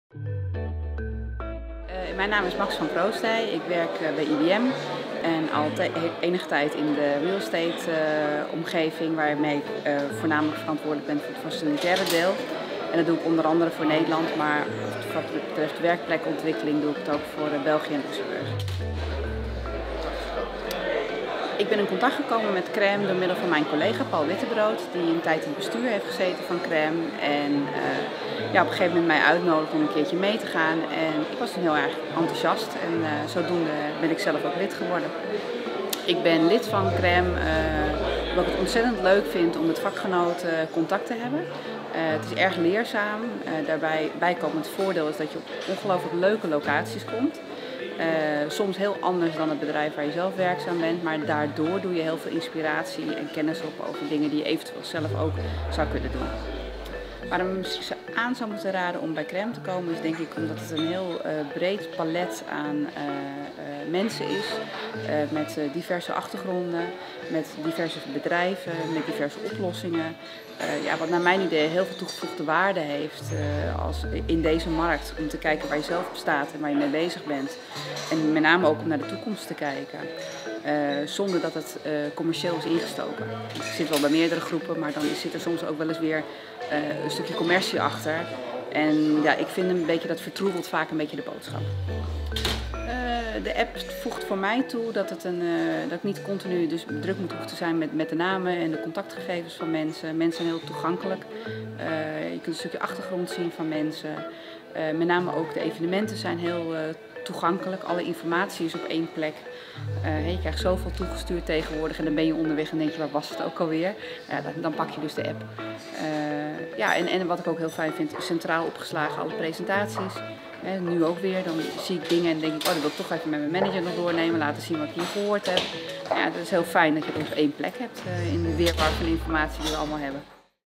Max van Proosdij (IBM) over CREME Nederland
In deze film geeft Max van Proosdij, Contract Manager Facility bij IBM, aan waarom zij lid is geworden van CREME Nederland; dé onafhankelijke eindgebruikersvereniging voor Corporate Real Estate Managers die zich bezighouden met huisvesting/vastgoed op strategisch niveau.